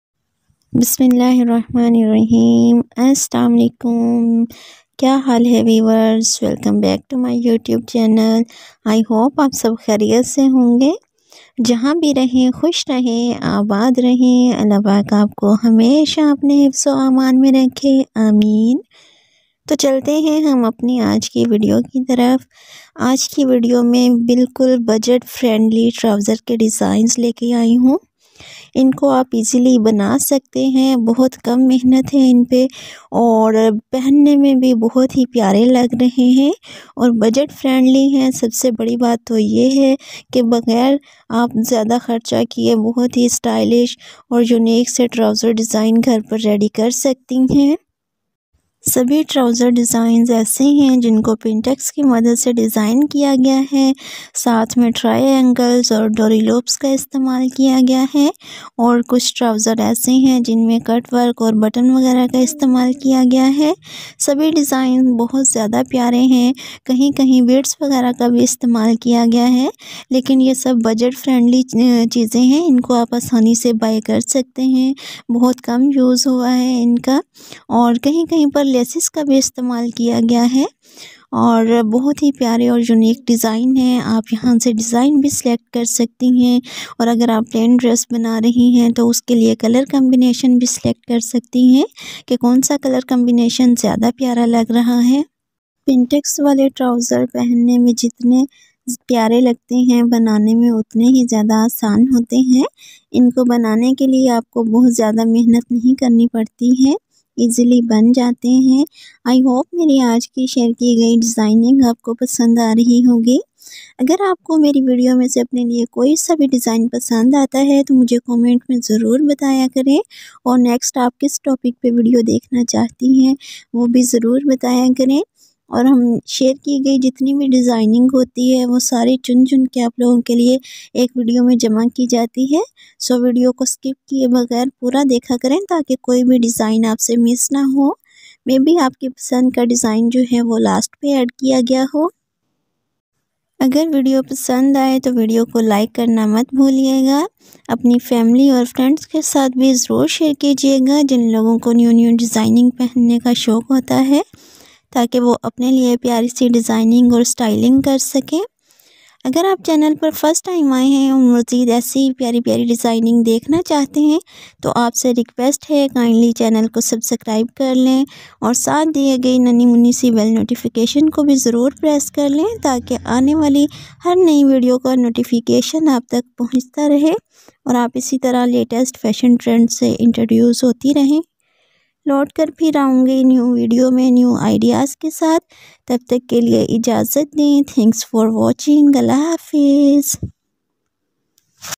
25 +Very Stylish Trouser Designs 2024 | Trouser Design With Pintex | Trouser Ke Design 2024
25 +Very Stylish Trouser Designs 2024 | Trouser Design With Pintex | Trouser Ke Design 2024

In this video I have shared latest, new and beautiful trouser paincha designs with pintex/pintucks.You can make these designs on your trousers in winter and summer.

New Trouser Paincha Designs 2024 For Winter